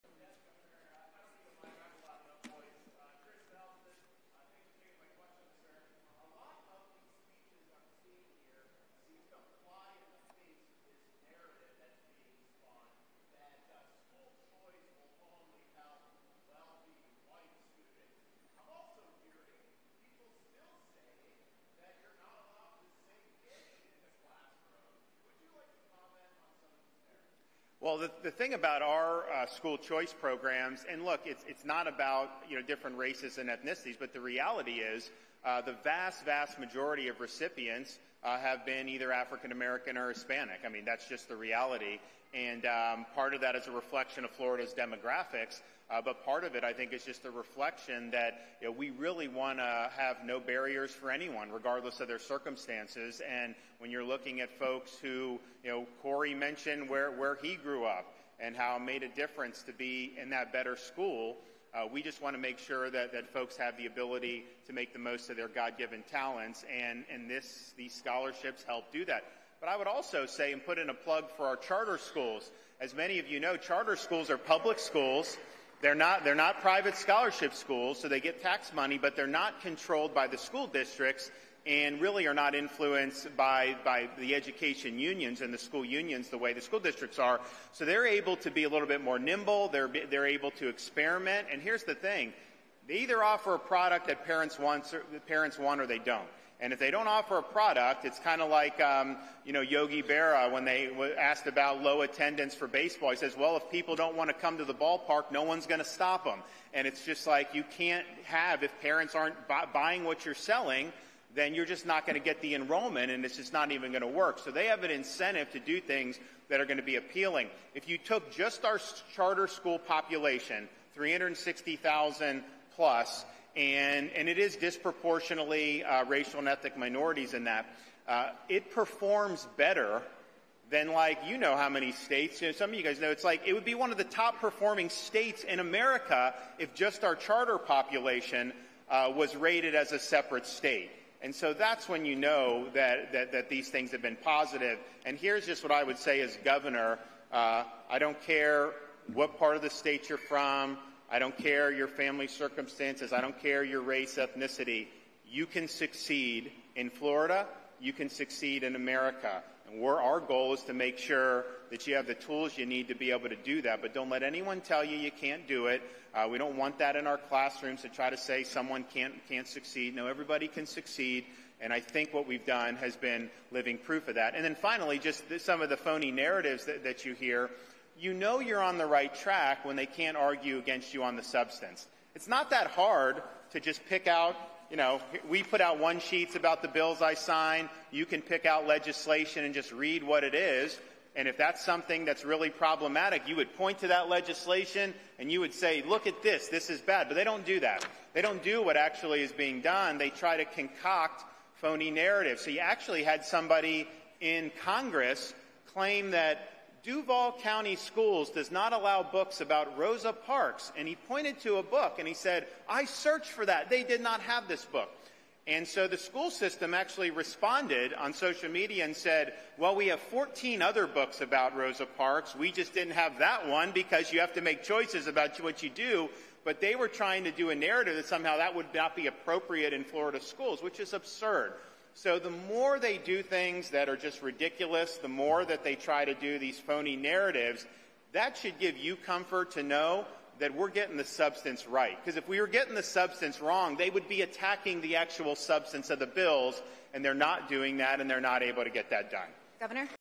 JUST IN: DeSantis Fires Back At Democrats' Claim Rosa Parks Book Was Banned From Florida School
(Editor's Note: Year should read 2023) At a press briefing promoting school choice, Gov. Ron DeSantis (R-FL) fired back at a Democratic claim that a book on Rosa Parks was banned from a Florida school.